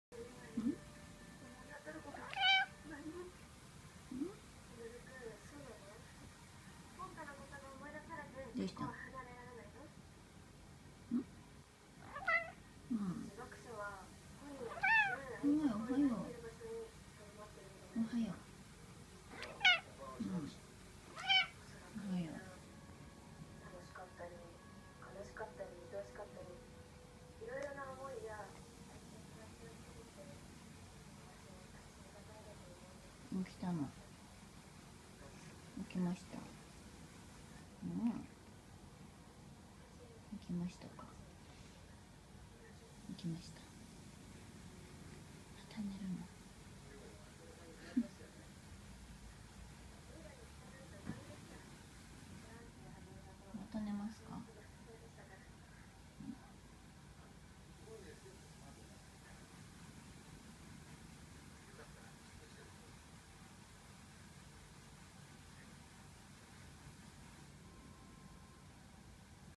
ノルウェージャン五右衛門。お喋り。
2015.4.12
よくしゃべるお猫さん。